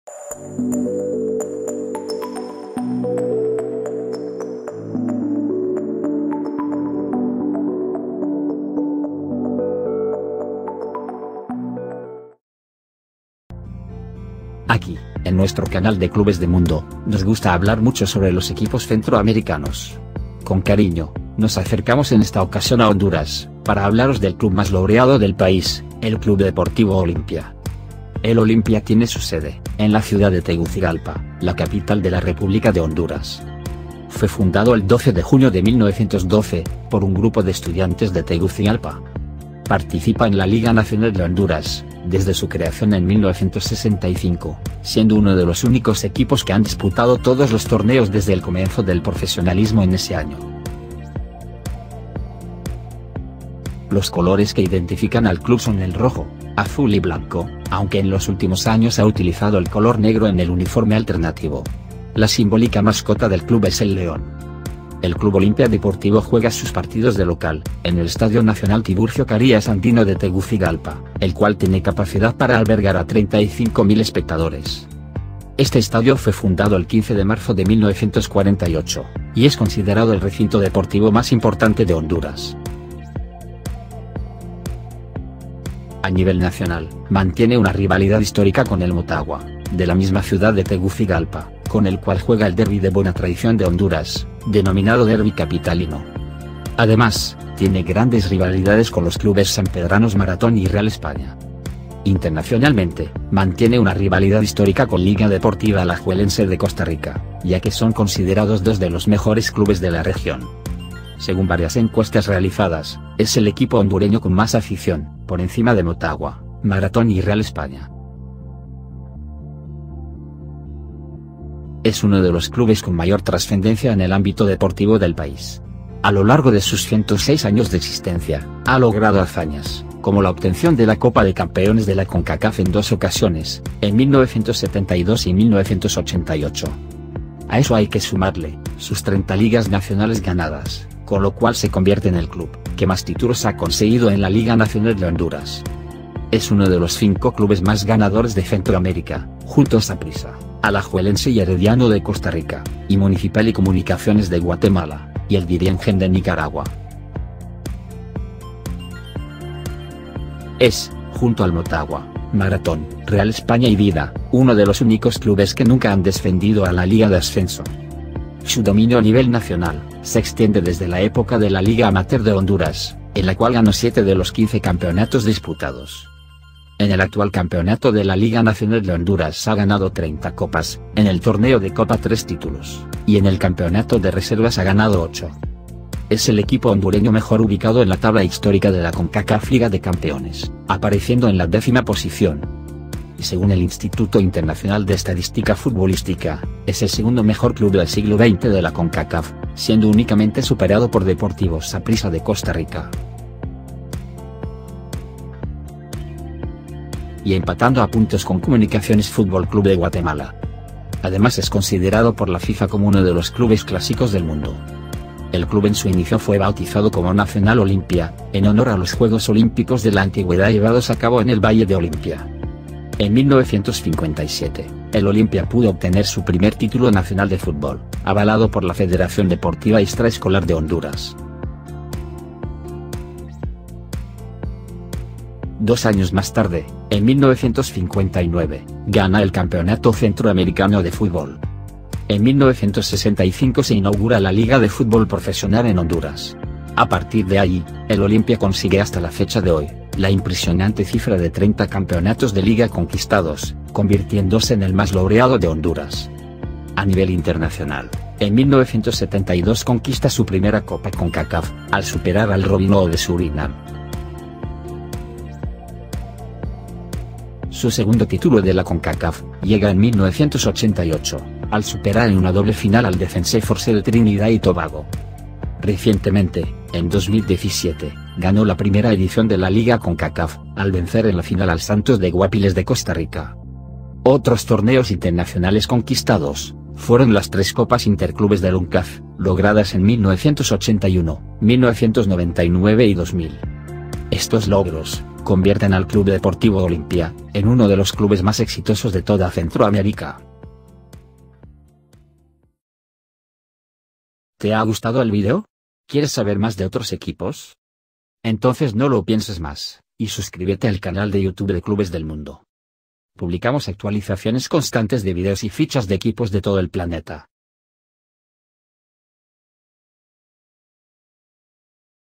CLUB OLIMPIA - El León, Rey de Honduras - Clubes del Mundo
El Club Olimpia es el club más laureado en Honduras.
Desde la implantación del Futbol Profesional en Honduras, el Olimpia ha ganado 30 campeonatos de Liga, mas que nadie.
Además, logro ganar 3 Copas de Honduras.
A nivel Internacional, ha ganado 2 Copas de Concacaf en 1972 y 1988.  También tiene 3 Interclubes Concacaf logradas en 1981, 1999 y 2000.
Recientemente han ganado en 2017, la primera edicion de la Liga Concacaf. Son uno de los clubes más exitosos de toda Centroamerica.
Se les conoce tambien con los apodos de El León, Los Albos o Los Melenudos.